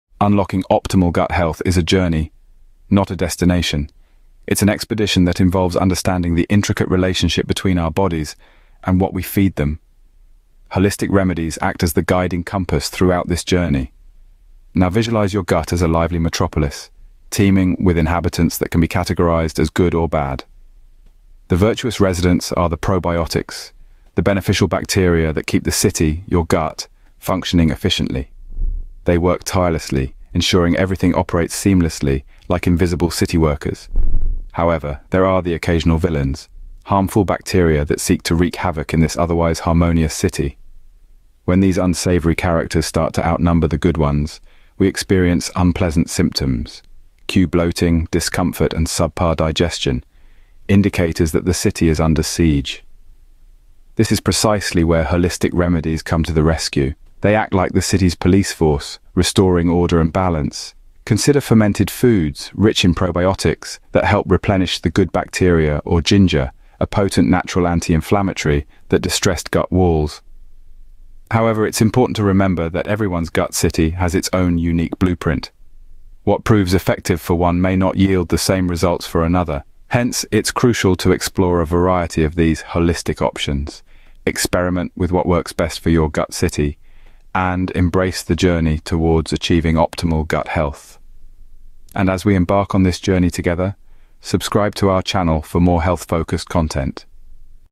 Transform Your Gut Health: Discovering Game-Changing Holistic Remedies (2024)
Did you know that your gut is home to trillions of bacteria?

Say goodbye to bloating and digestive issues forever with these game-changing holistic remedies I'm about to share with you!

Learn about natural solutions and practices to support your digestive system and improve overall well-being.

 Unveil the best tips and techniques to achieve a healthier gut in 2024 and beyond.

Dive into this video to revolutionize your approach to gut health and embrace a holistic lifestyle.

As the bulk of my video audience reside in North America, effective from 15 May 2024, a new video will be launched daily at 19.00 New York time.

Hardback - £10.00/€15.00/$15.00 (US)E